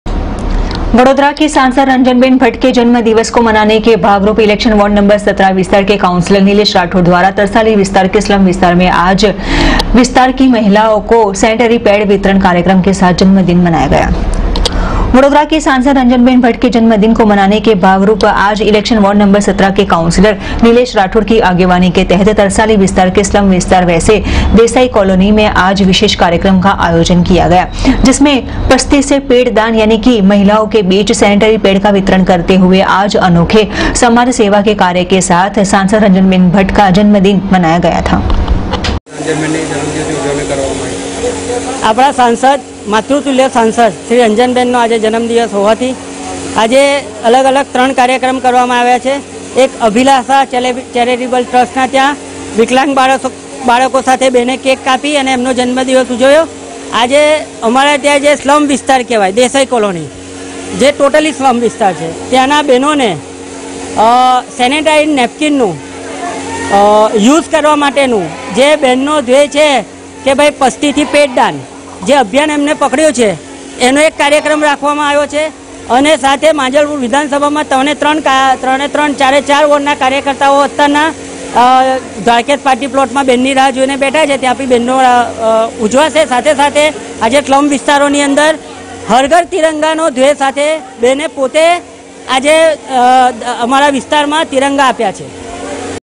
ibn news Vadodara
सांसद रंजन बेन भट्ठ के जन्मदिन के उपलक्ष में इलेक्शन वॉड नंबर 17 तरसाली विस्तार में मनाया गया कार्यक्रम